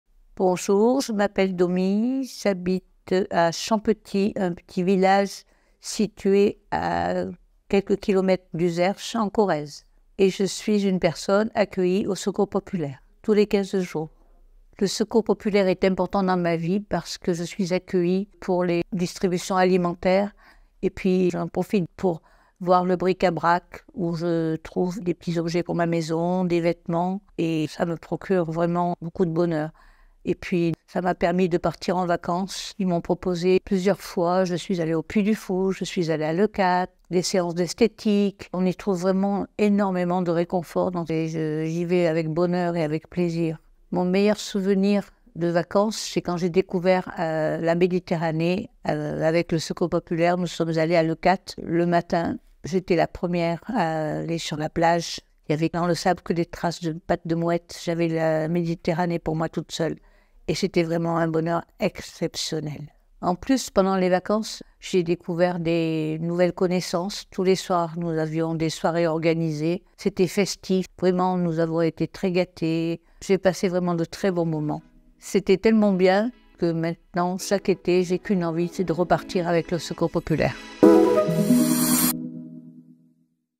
"Mon meilleur souvenir, c'est la découverte de la Méditerranée" Témoignage de Dominique
« Mon meilleur souvenir, c'est la découverte de la Méditerranée »

C’est le sentiment que Dominique a ressenti lors de son séjour dans le sud de la France. Personne à la fois aidée et bénévole, le lien qu’elle entretient avec notre association dure depuis plusieurs années et l’aide à affronter les difficultés du quotidien.

Rejoignez-nous ou faites un don !